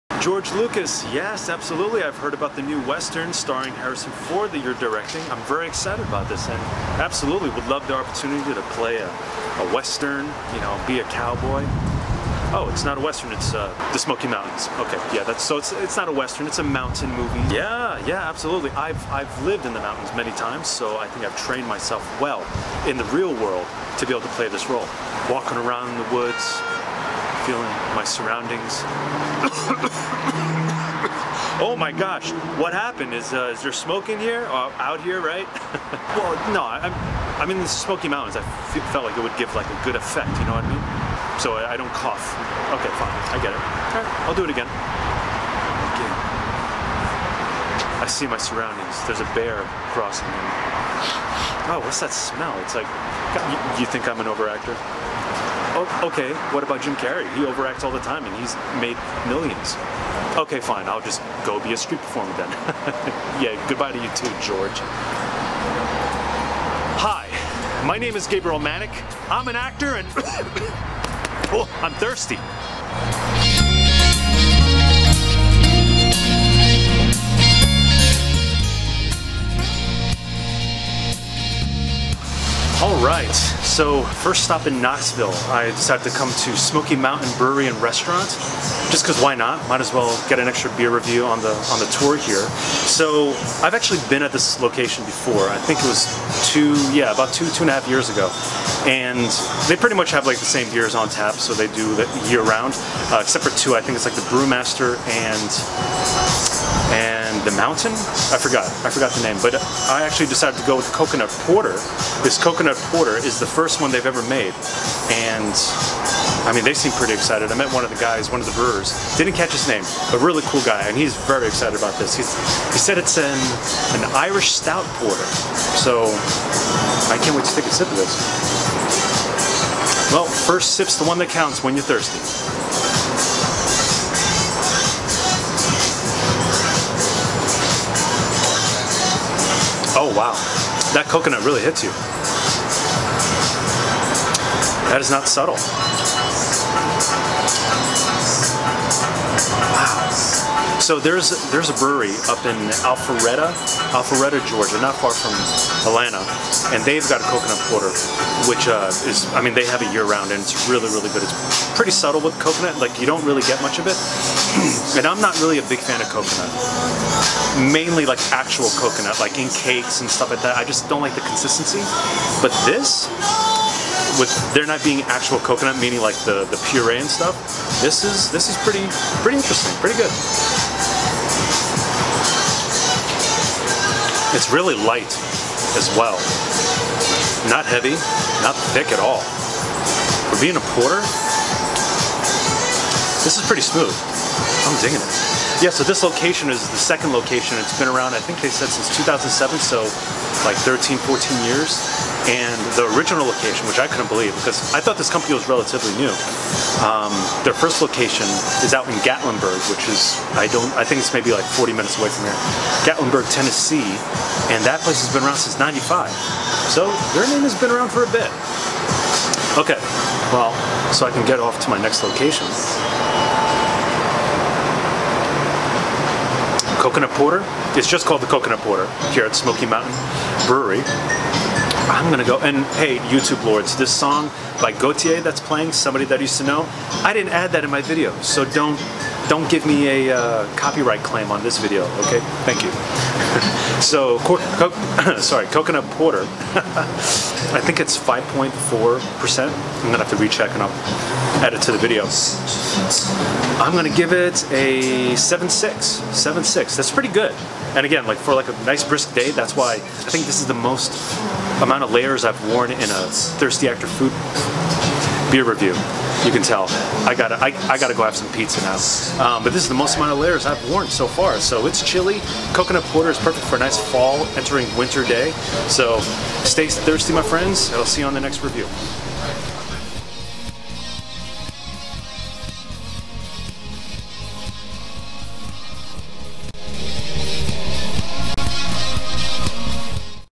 I Called GEORGE LUCAS For A Role In His Next Movie | Smoky Mountain Brewery (Knoxville, TN)
First stop in Knoxville takes me to Smoky Mountain Brewery.

Beer Under Review:
Coconut Porter

Smoky Mountain Brewery
11308 Parkside Drive